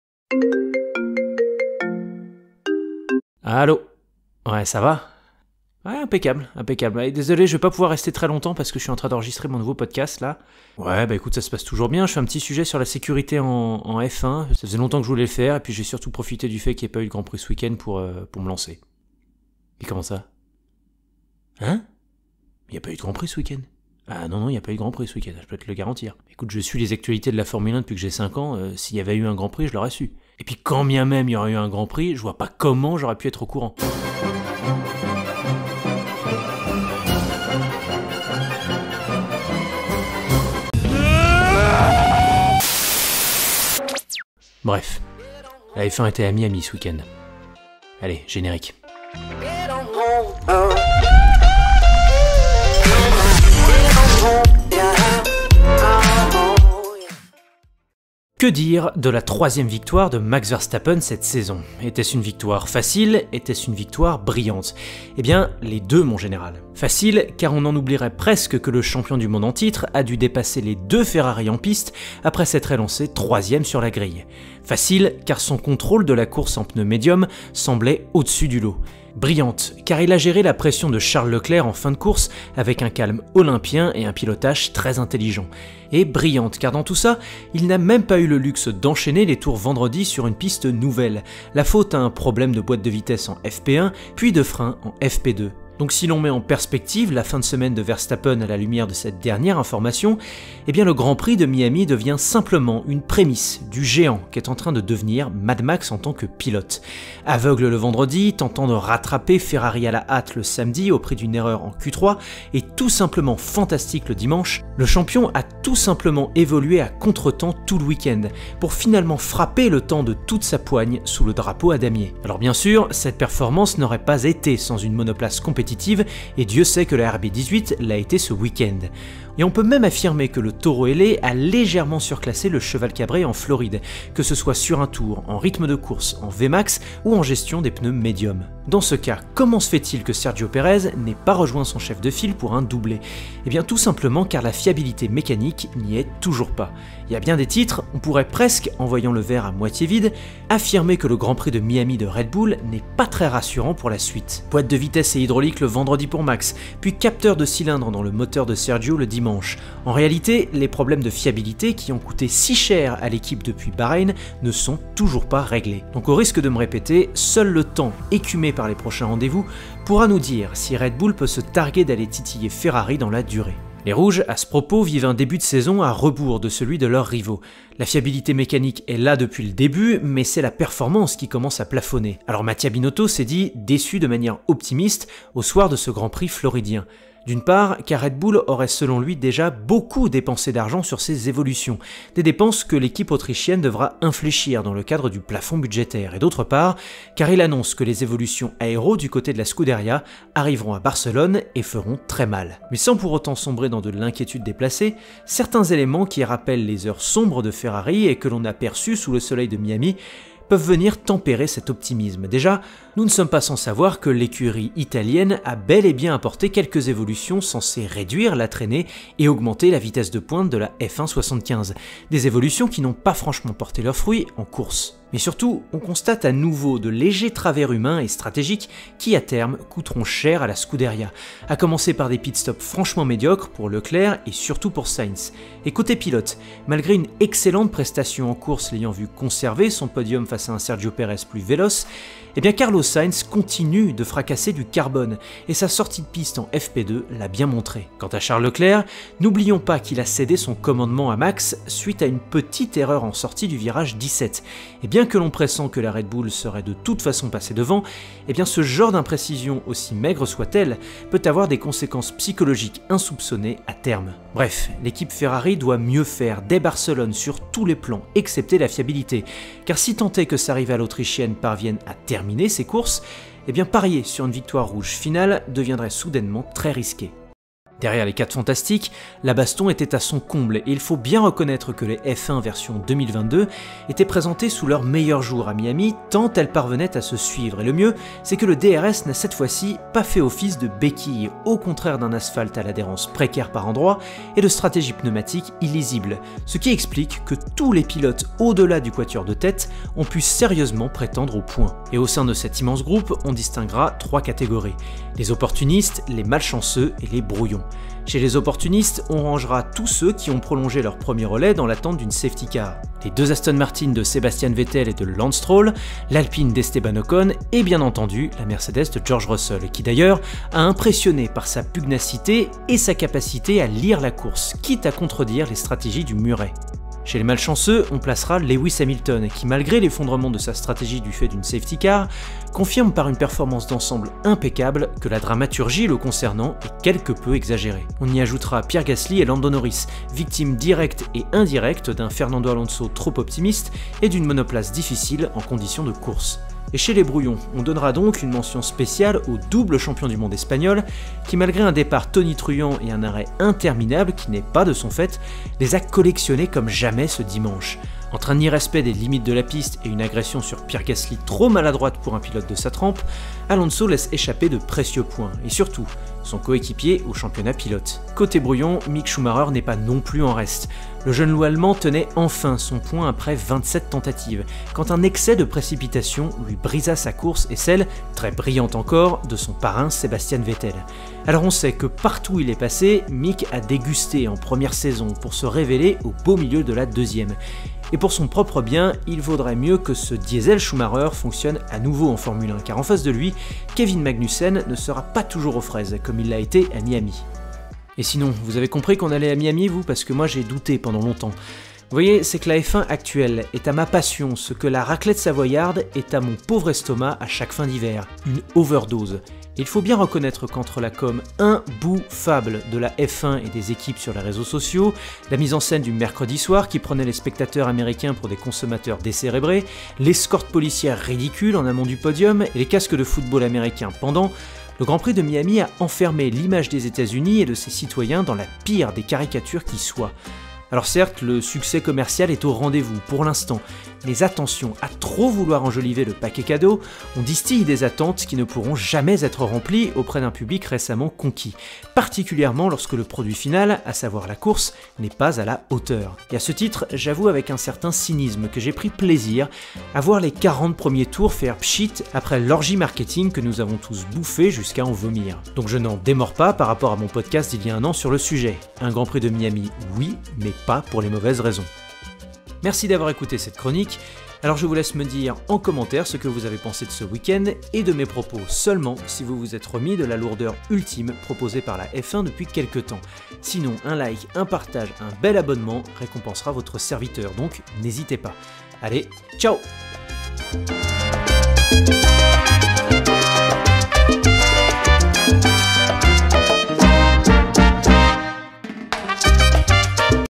FERRARI DOIT RÉAGIR - Chronique F1, GP de Miami 2022
Le week-end de RedBull et Max Verstappen à Miami est-il brillant ou inquiétant ? Pourquoi la Scuderia Ferrari, Charles Leclerc et Carlos Sainz doivent-ils impérativement réagir ? Votre serviteur est-il sujet à une overdose de F1 ?

0:00 - Allô ?
0:58 - Verstappen brillant, Red Bull inquiétant
2:53 - Ferrari doit hausser son niveau de jeu !
4:37 - Miami : les opportunistes, les malchanceux et les brouillons
6:36 - Overdose
7:42 - Conclusion
Sources musicales
Vodovoz Music Productions
La plupart des photos utilisées proviennent de la source suivante : Race Fans
N'hésitez pas à liker (si vous aimez), partager et surtout réagir : votre avis m'intéresse ! A bientôt pour un nouveau podcast de F1 à la Loupe :)